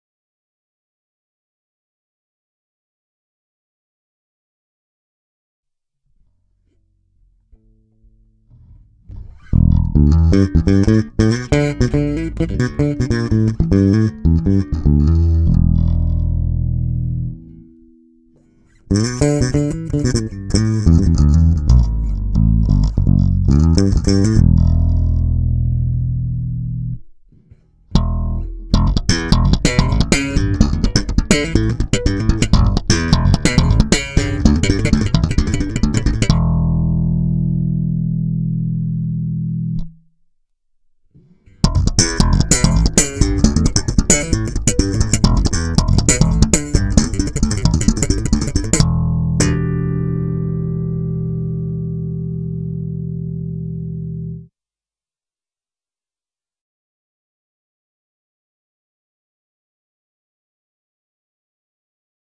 Boss EH-2 Enhancer on Bass
Boss EH-2 Enhancer played on custom made bass with swamp ash body, maple neck, ebony fretboard, EMG 35DC pickups, EMG BTC Control preamp.